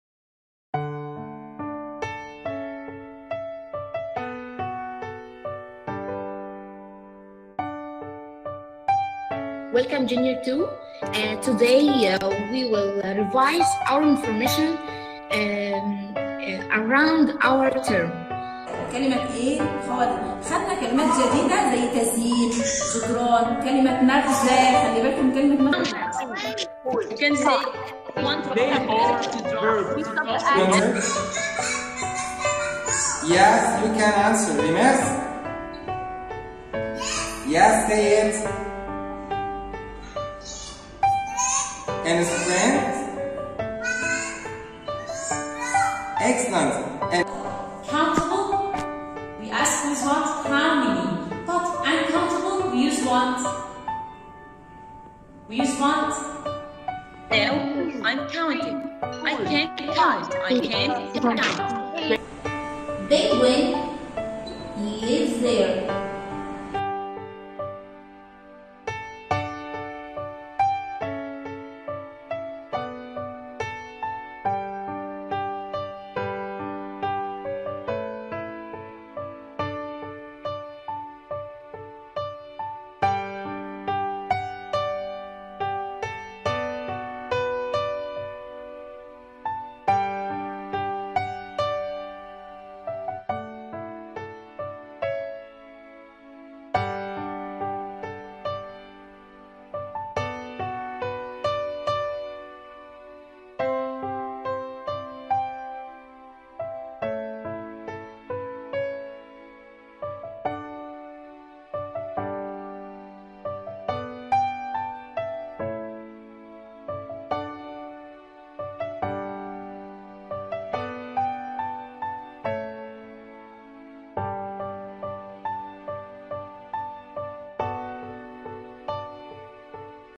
الاسبوع الاول من تطبيق winjigo
نشكر ابنائنا وأولياء الأمور الاعزاء ومعلمينا الافاضل على المجهود المبذول لإنهاء متطلبات التعلم بامتاع على التطبيقwinjigo استمتعنا معا بالاسبوع الاول
والى لقاء متجدد بالاسبوع الثاني
ejs_borg1